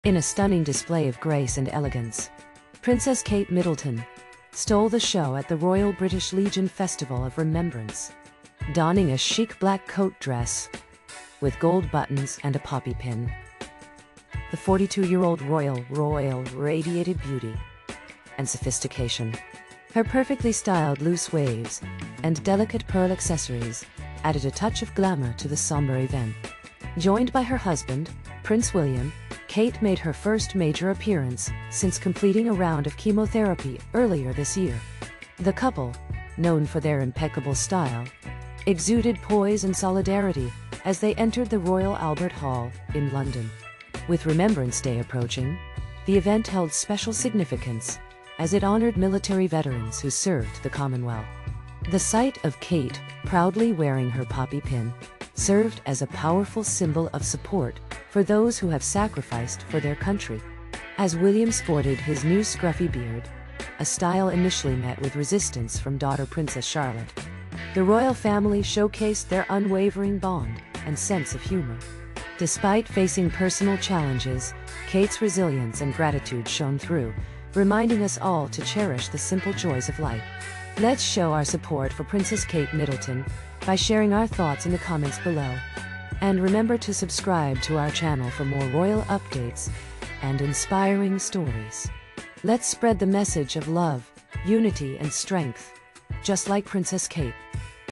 Kate Middleton's Stunning Black Dress at Festival of Remembrance
Princess Kate Middleton makes a glamorous appearance in a black dress at the Royal British Legion Festival of Remembrance after completing chemotherapy. Joining Prince William, Kate's outfit and presence steal the show, paying tribute to military veterans with her elegant look. Learn more about Kate's journey and how she has navigated through challenging times with strength and grace in this insightful video.